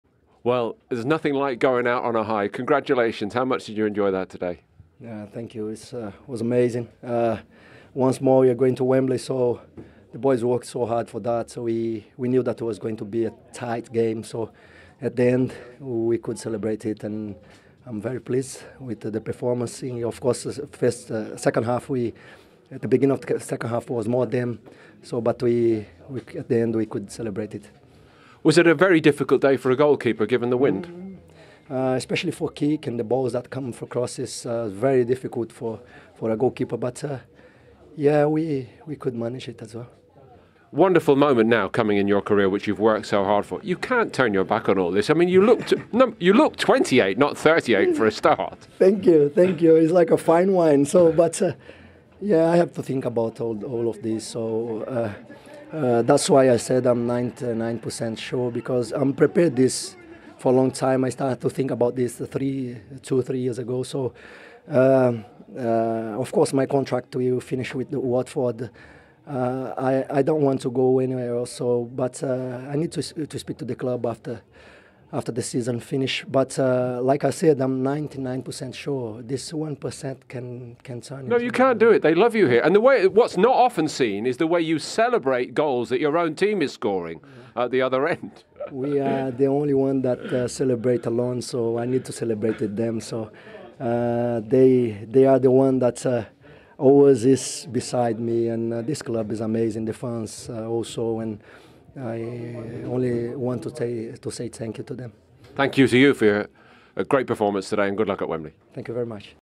"I'm like a fine wine!" Heurelho Gomes has a message for Watford fans after his last home game
Heurelho Gomes spoke after what could potentially be his last game at Vicarage Road, with the Watford goalkeeper showing his emotions after the final whistle.